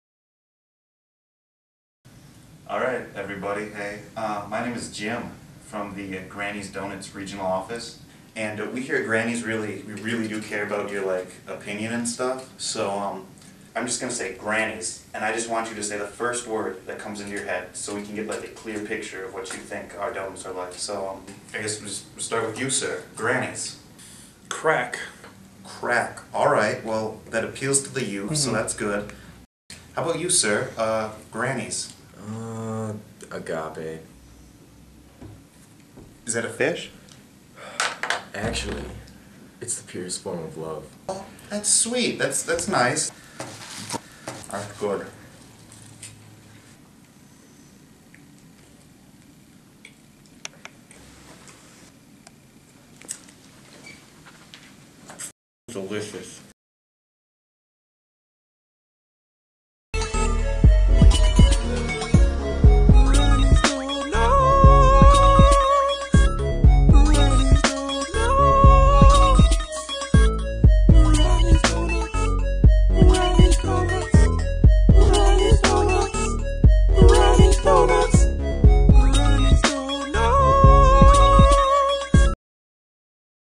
Granny's Donuts Advertisement
An advertisement for the amazing donut shop Granny's Donuts located in West St. Paul, MN